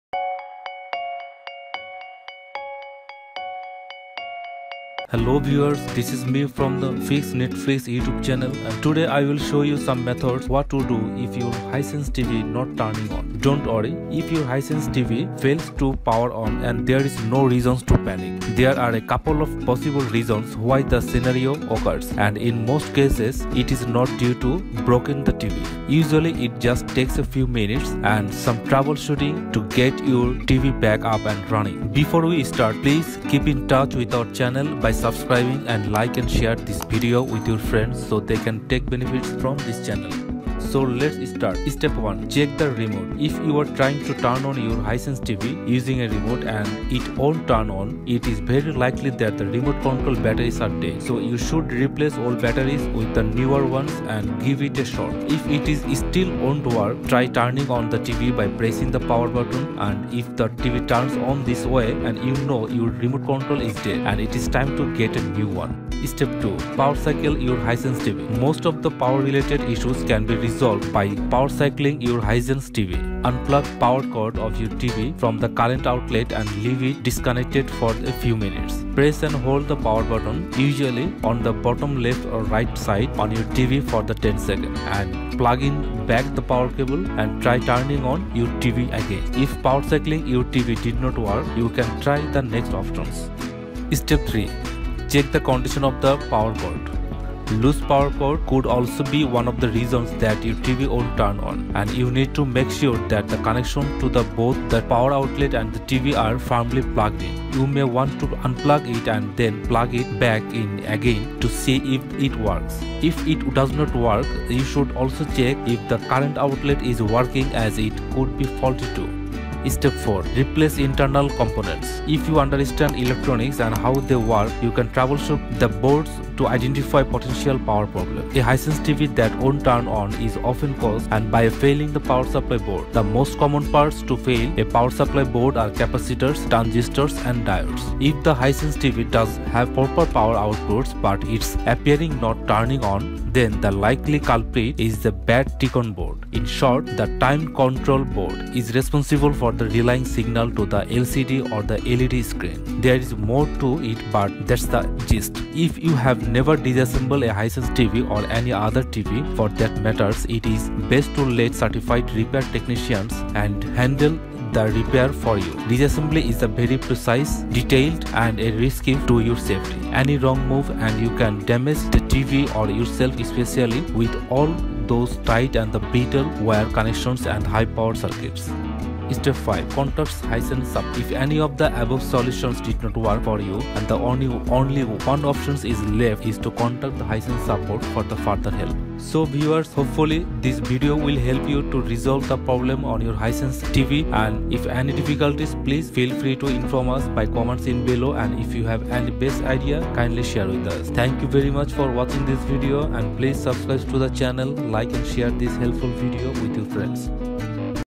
Hisense TV Won't Turn On / Red Light Flashes / No Red Light / Complete Fix Guide
If your Hisense TV not turning on and you’re out of ideas, in this video you’ll discover all the possible causes and their solutions. Complete Fix Guide